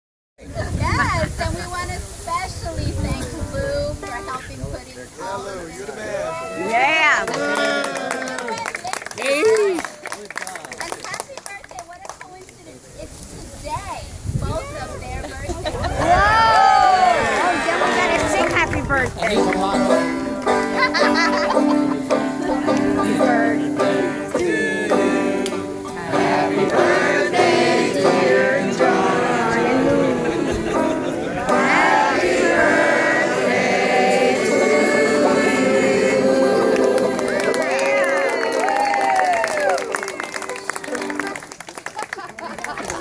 Happy Birthday to John McEuen and Lou the Chef!
John McEuen's home is affectionately referred to as Casa McEuen. It's a beautiful property where a river really does run through it (well, alongside it anyway). It's a beautiful blessed land home to fox, raccoon, snake, squirrel, crows, turkey hawk, moles, cardinals, finches, doves and pigeon. On the water side there are any number of ocean fish species along with manatee, alligator and shark. to hear more John McEuen music...